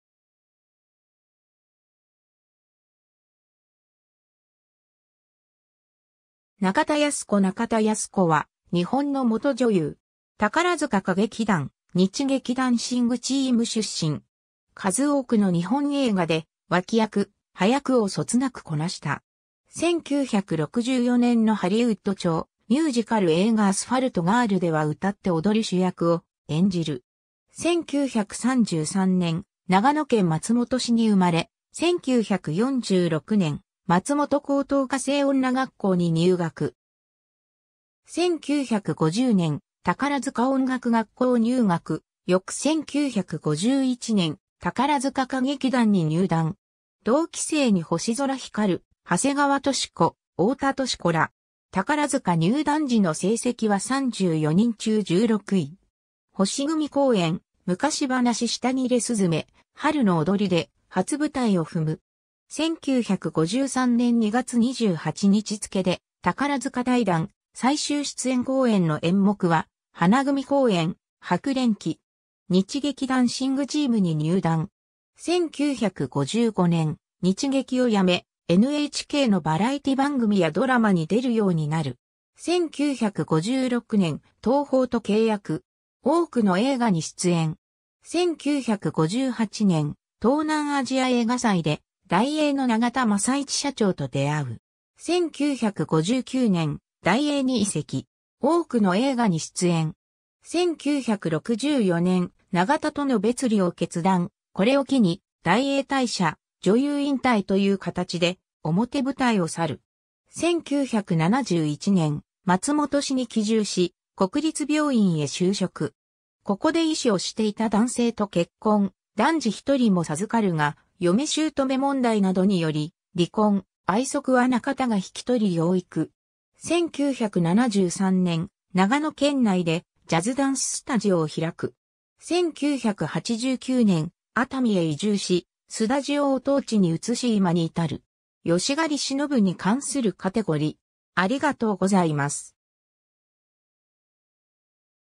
中田康子
中田康子（1956年ごろ） 中田 康子（なかだ やすこ、1933年2月18日 - ）は、日本の元女優。
宝塚歌劇団・日劇ダンシングチーム（NDT）出身。
数多くの日本映画で脇役、端役をそつなくこなした。
1964年のハリウッド調ミュージカル映画アスファルト・ガールでは歌って踊る主役を演じる。
 1933年、長野県松本市に生まれ、1946年、松本高等家政女学校に入学。
 1950年、宝塚音楽学校入学、翌1951年、宝塚歌劇団に入団。
同期生（38期）に星空ひかる、長谷川季子、太田淑子（若樹美乃里）ら。
宝塚入団時の成績は34人中16位。
星組公演『昔噺舌切雀/春のおどり』で初舞台を踏む。
 1953年2月28日付で宝塚退団、最終出演公演の演目は花組公演『白蓮記』。
日劇ダンシングチーム（NDT）に入団。
 1955年、日劇を辞め、NHKのバラエティ番組やドラマに出るようになる。
 1956年、東宝と契約。
多くの映画に出演。
 1958年、東南アジア映画祭で大映の永田雅一社長と出会う。
 1959年、大映に移籍。
 1964年、永田との別離を決断、これを機に大映退社・女優引退という形で表舞台を去る。
 1971年、松本市に帰住し、国立病院へ就職。
ここで医師をしていた男性と結婚・男児ひとりも授かるが嫁姑問題などにより離婚、愛息は中田が引き取り養育。
 1973年、長野県内でジャズダンス・スタジオを開く。
 1989年、熱海へ移住し、スダジオを当地に移し今に至る。
 由狩しのぶに関するカテゴリ: